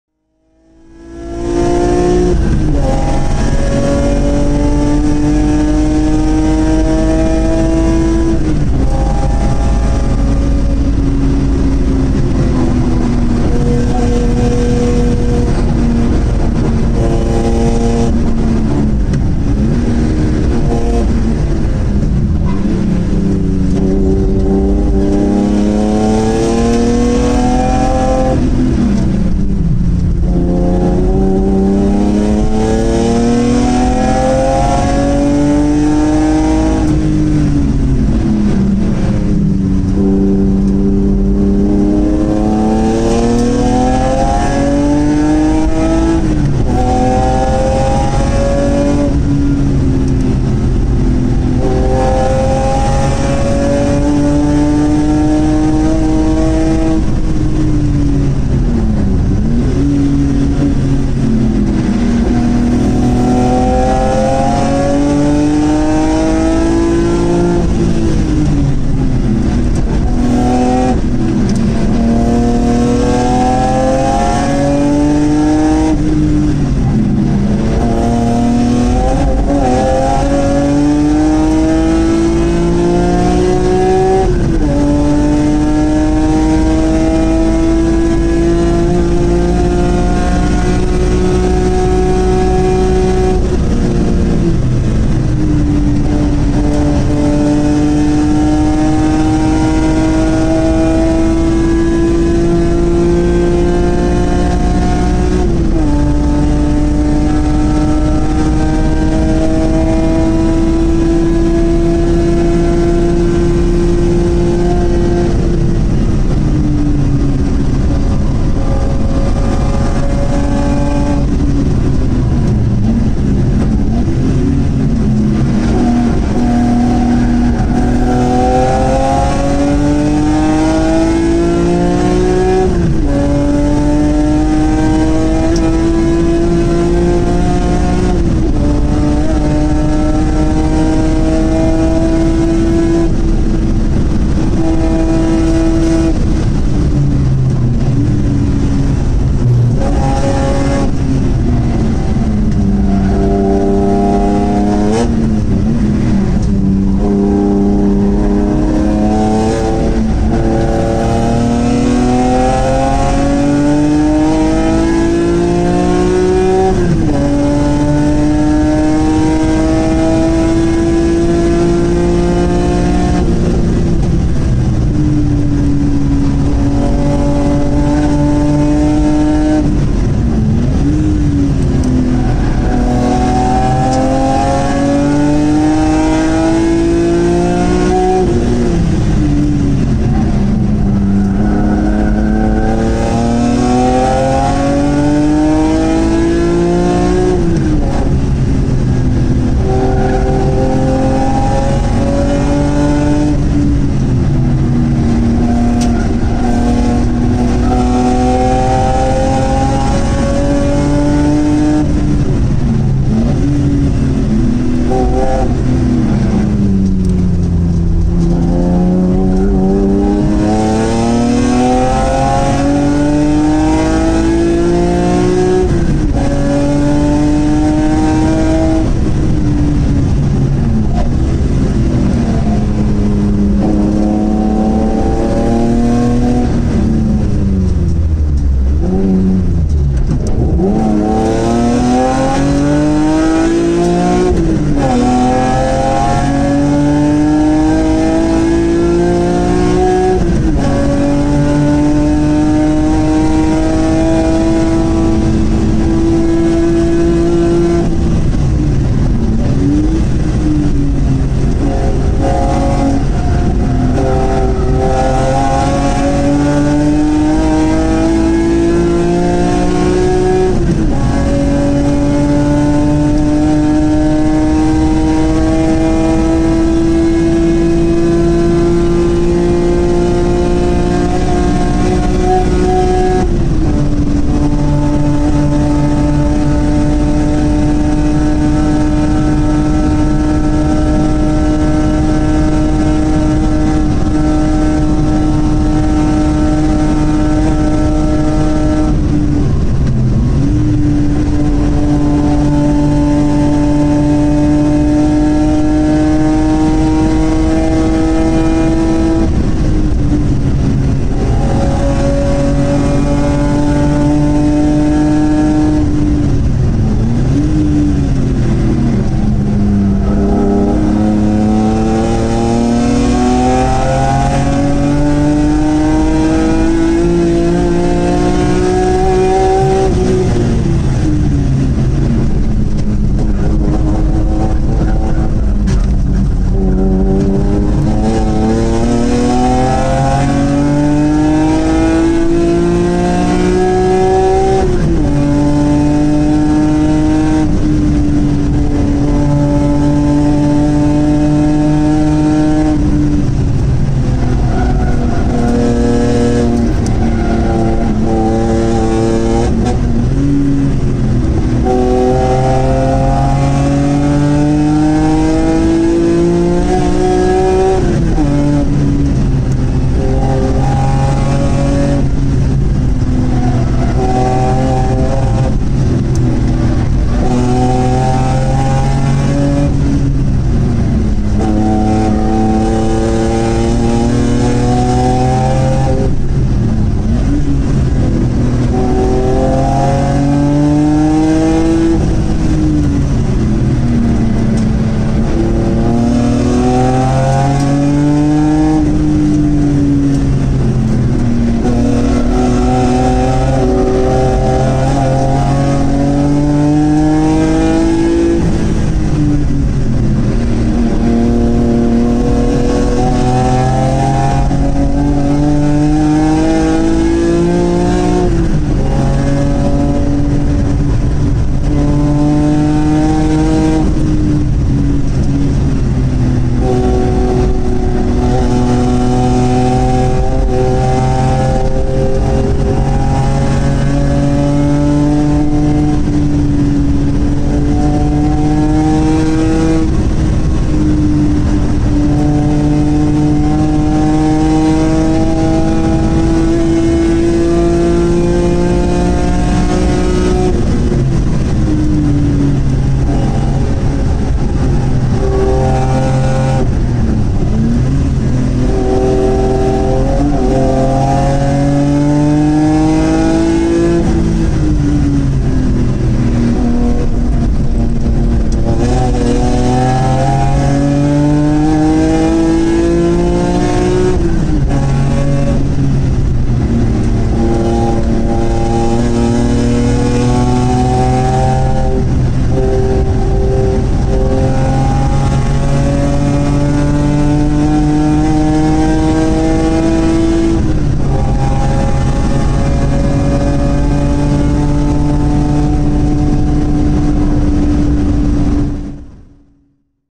BMW e36 Compact 332ti 8.09 on Nurburgring
my  fifth lap 2019 and im feeling a lot of emotion sickness here.